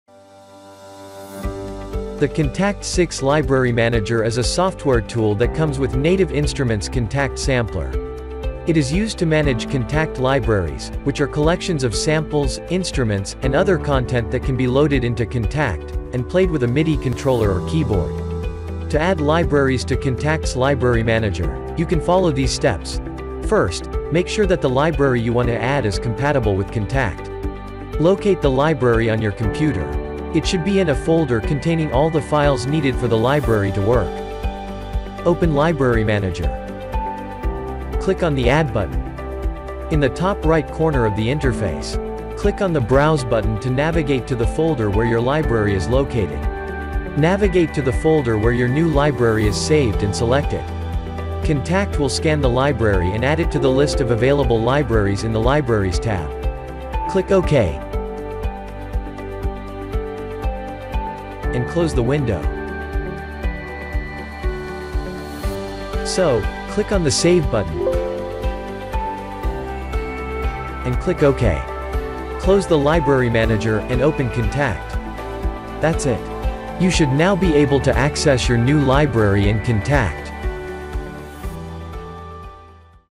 Kontakt 6 - How To Add Libraries In Kontakt's Library Manager
The Kontakt 6 Library Manager is a software tool that comes with Native Instruments' Kontakt sampler.

It is used to manage Kontakt libraries, which are collections of samples, instruments, and other content that can be loaded into Kontakt and played with a MIDI controller or keyboard.